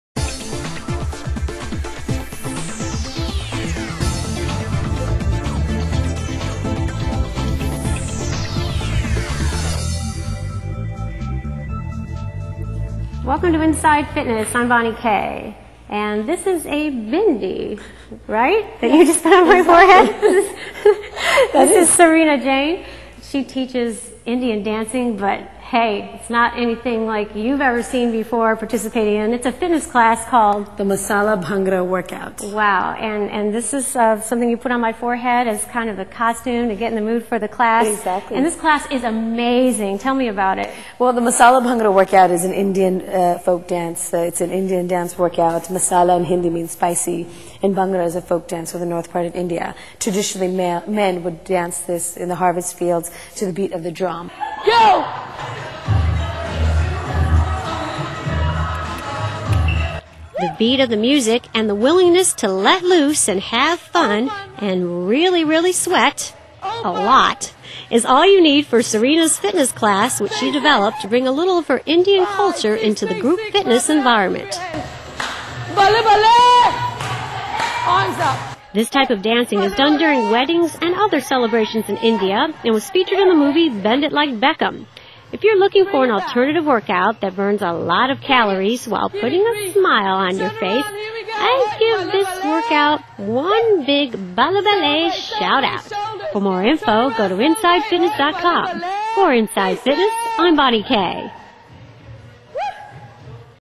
Inside Fitness -Indian Dance workout
Bonnie Kaye shows you a fun, exciting new workout that will make you feel like your in a Ballywood film!